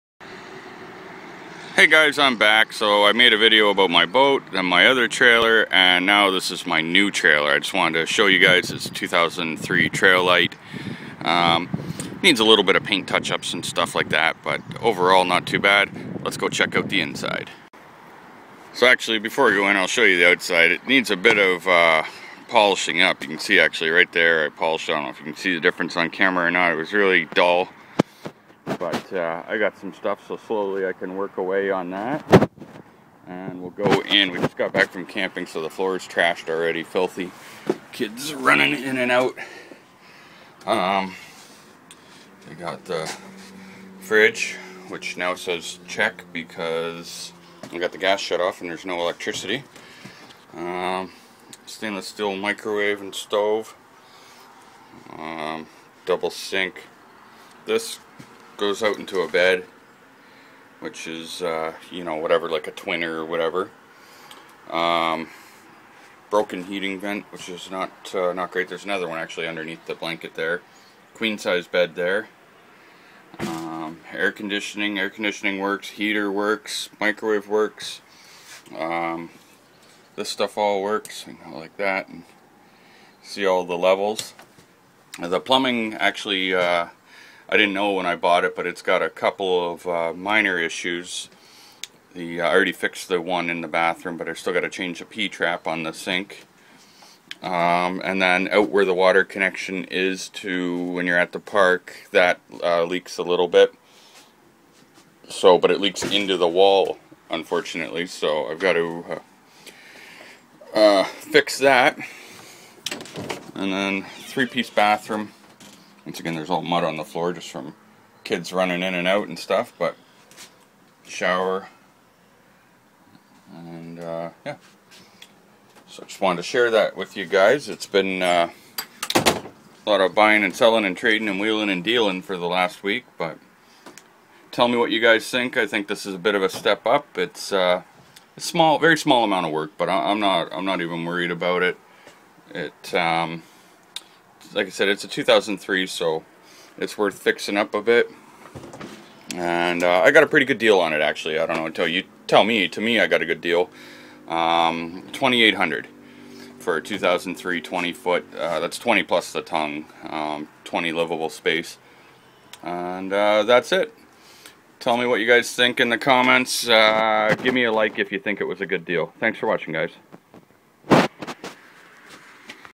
If the trailers a rock'n, don't come a knock'n
Today I'm showing you all my new (to me) 2003 Trail Lite by R Vision. It is 20' pull behind camper. We were really looking for an RV, however came into this deal so just went with it. It needs a few little touch ups here and there, but overall not too bad.